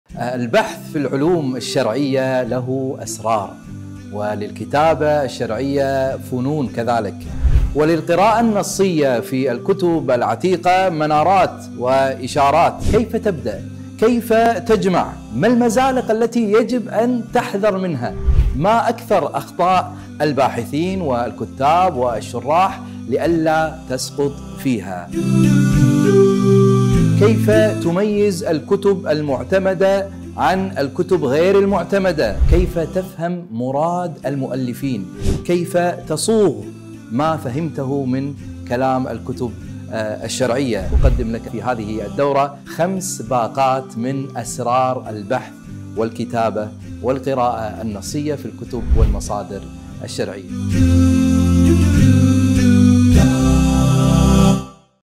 أسرار البحث والكتابة الشرعية | أ.د. ياسر النشمي | كورس ستور
للقراءة في كتب التراث الإسلامي وللكتابة العلمية في التخصصات الشرعية؛ أسرار وأدوات ومنهجيات📚

أ.د. ياسر عجيل النشمي

📕 المميزات
✔️ شهادة إتمام الكورس
✔️ الكورس في حسابك دائمًا
✔️ المشاهدة في أي وقت ومن أي مكان
✔️ جودة ومعايير عالية